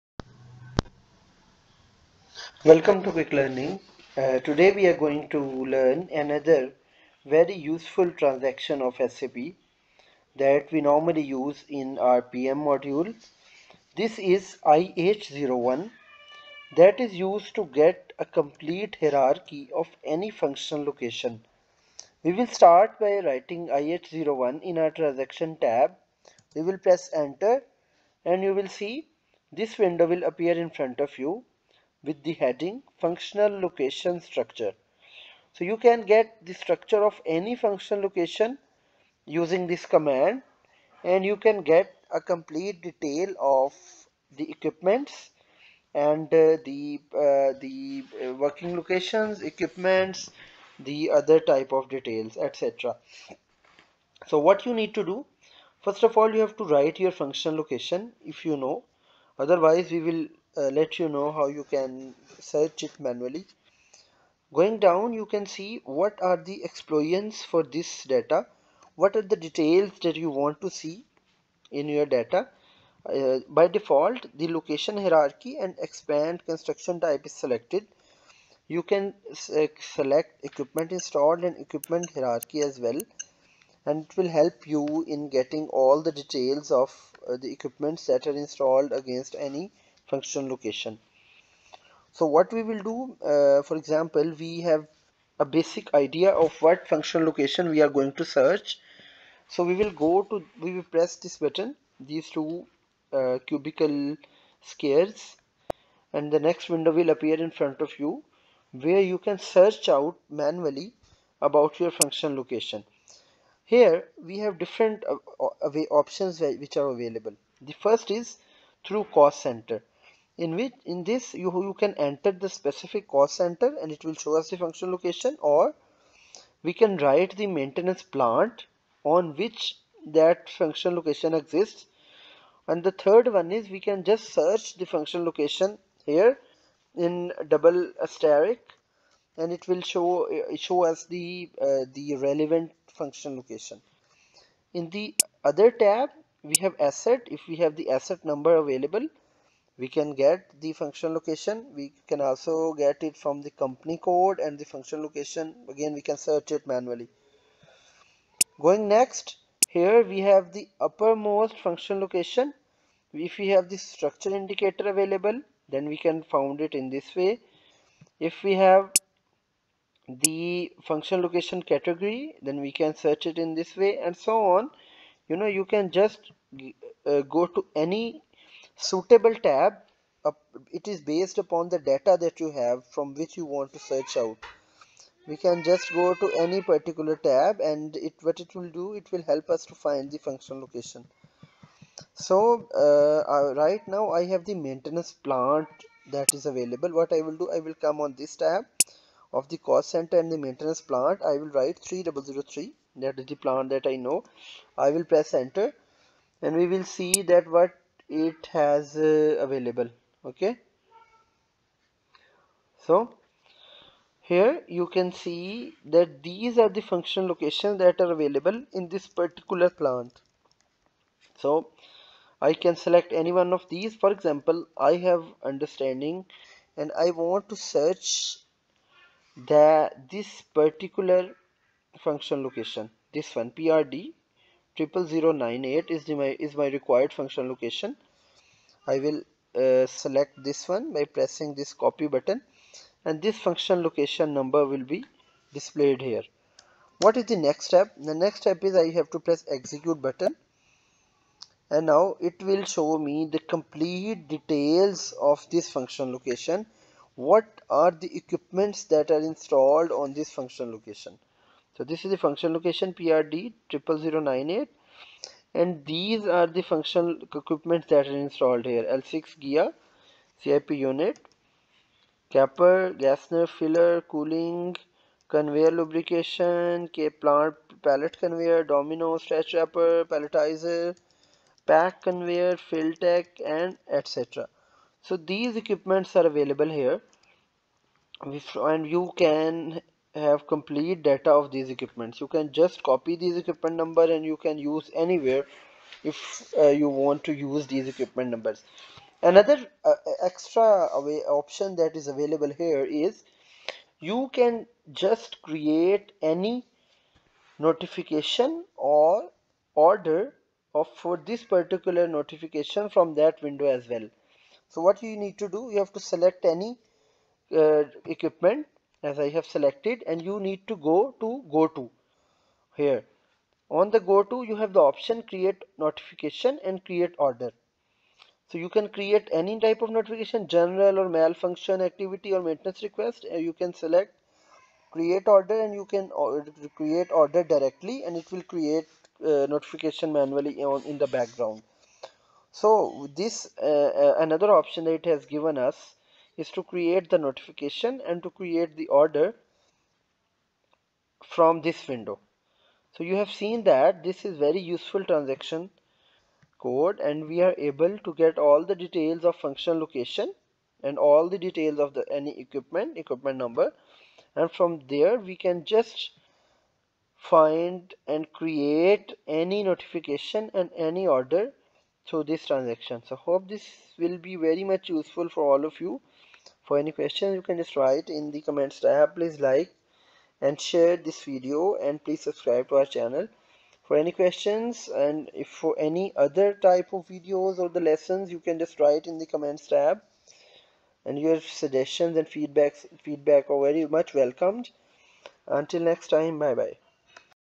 Check Functional Locations & Equipment in SAP | IH01 T-Code Tutorial | ERP Made Easy
In this video you will learn how to use IH01 to get all details of any functional location and its installed equipments.